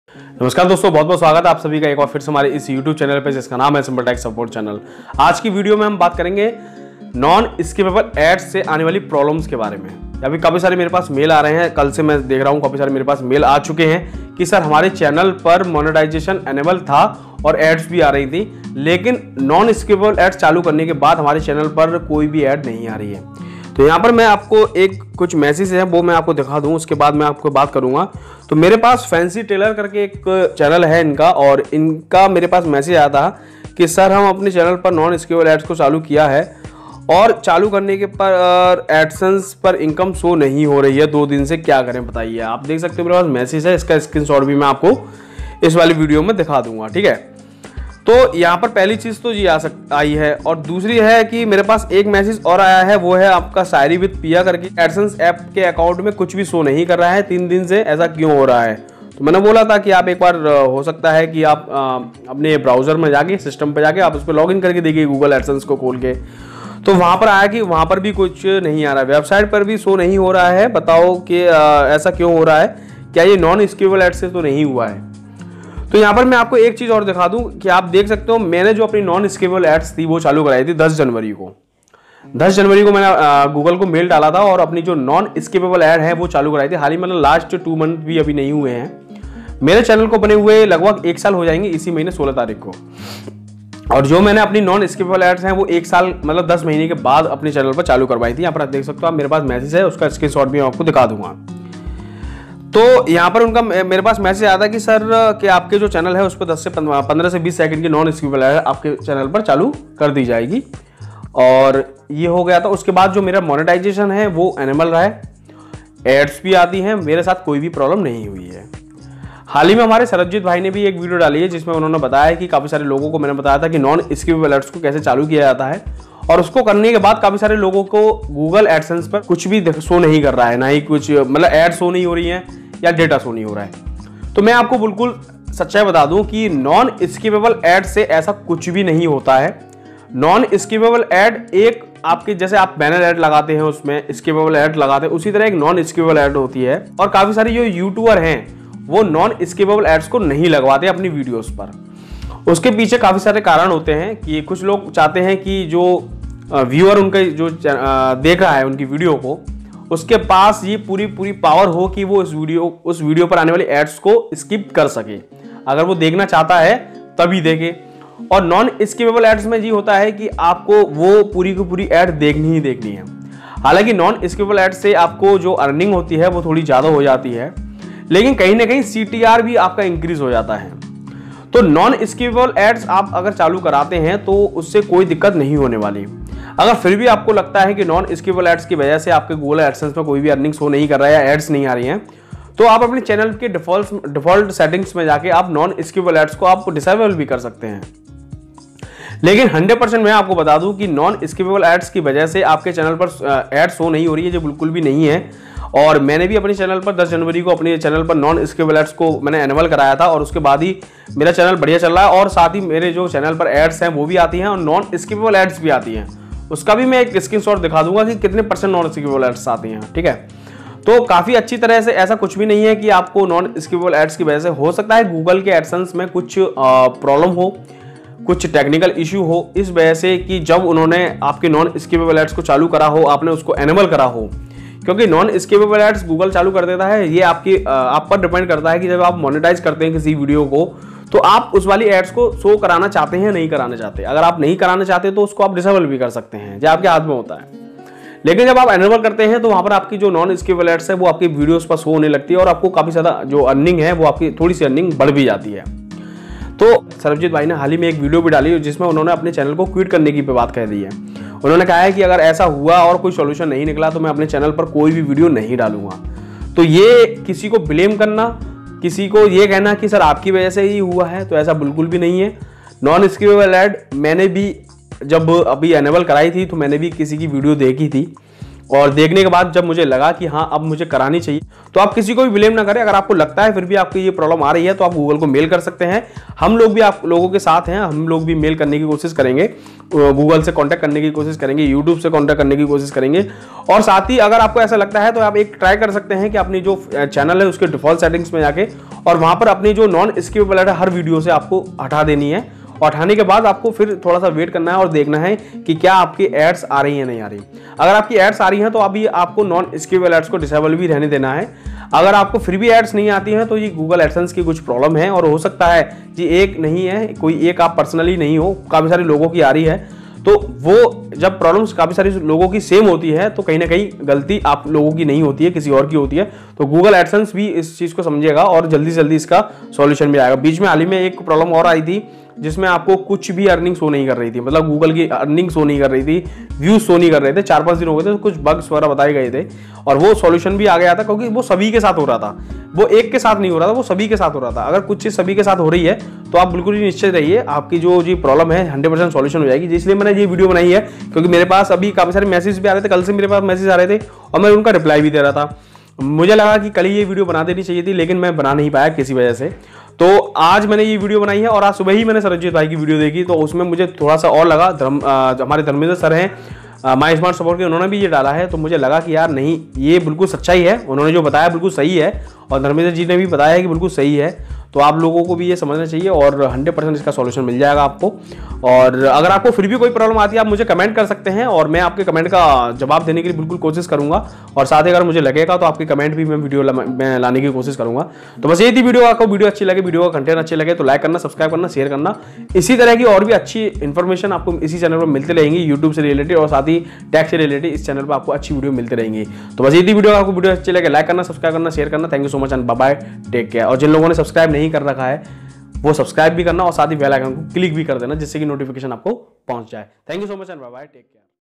Non Skippable ads problem ! pls don't quit youtube sharabjeetji (Aayiye sikhte hai)
Namaskar dosto is video me aapko bataunga ki aaj kal kafi logo ko problem aa rhi he google adsense se related aaj is video me baat karenge Non Skippable ads ke bare me ...